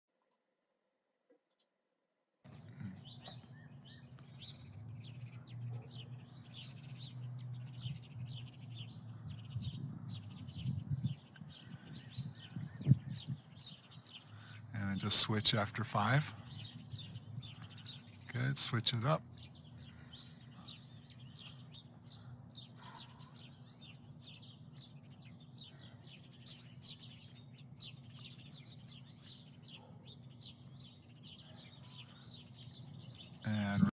A-Frame Upper Torso Rotation
This is a great middle spine (thoracic spine) rotational mobility exercise to gain both rotational flexibility and strength in the upper torso to improve your abillity as a golfer to rotate your upper torso independently frojm your lower torso. Upper/lower body separation helps with improving your coil and loading/unloading potential for improved swing power and distance.